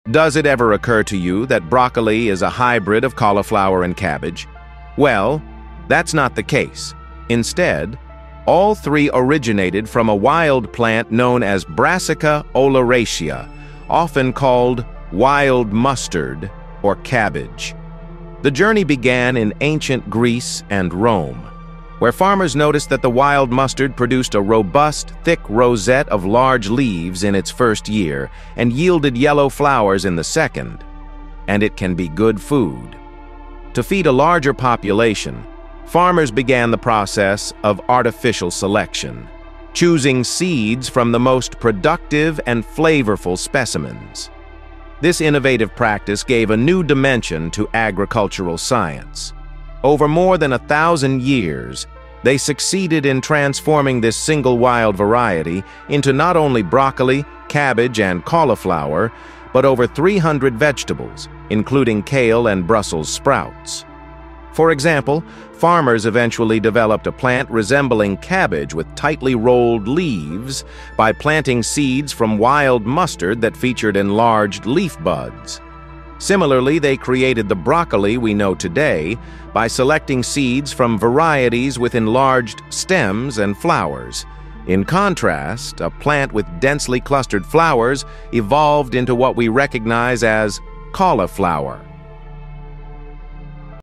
How is Broccoli 🥦related to cabbage and cauliflower?
We often have misconceptions about the origins of certain vegetables, and broccoli is one of them. Broccoli may be considered a man-made vegetable, as it did not exist on Earth until Roman and Greek farmers began experimenting with a wild variety of cabbage, also known as wild mustard. This video will explain the entire journey of broccoli, from its wild origins to its development as a modern vegetable.